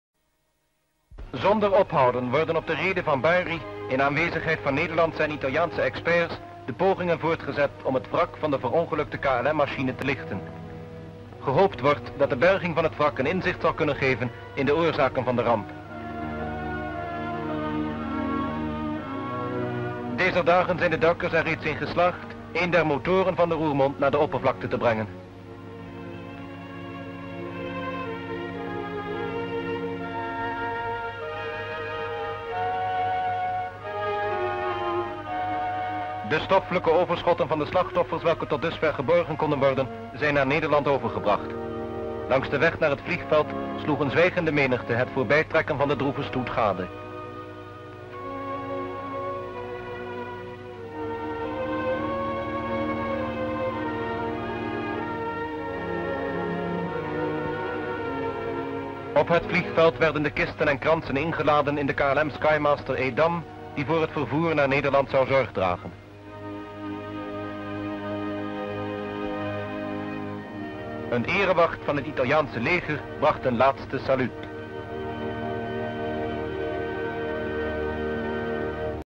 The salvage of the Roermond (Weeknummer 49-27) 🔴
Description (more): Flew on 23 June, during the flight, the KLM Constellation "Roermond" on fire and collapsed, just off the coast of Bari in southern Italy, in the sea. Images of the salvage and transport of the recovered victims to the airport for the flight by KLM plane to Netherlands. Shots:-Italian divers going into the place of the disaster water; -a plane engine is omhooggetakeld out of the water;  -a long line of freight cars with coffins in the flatbed will drive slowly through the streets of Bari, accompanied by carabinieri and followed by begrafenisgangers on foot under many public interest; -at the airport are the boxes transferred in a KLM Skymaster; a division of the Italian army presents the rifle.